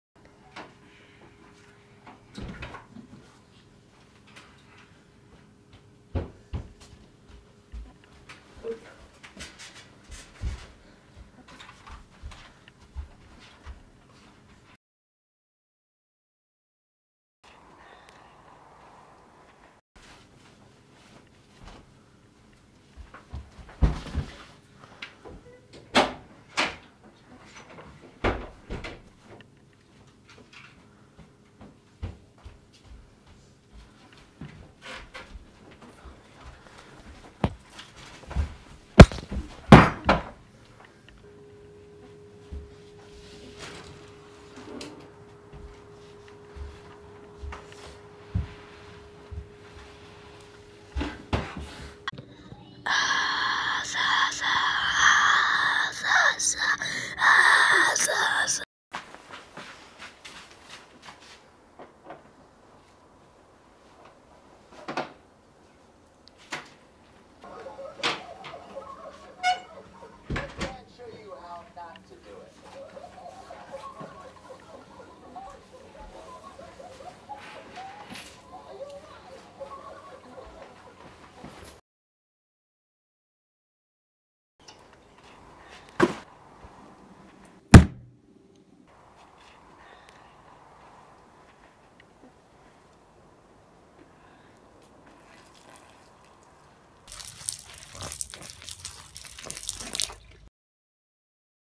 drömmen
en film om en man som drömmer framtiden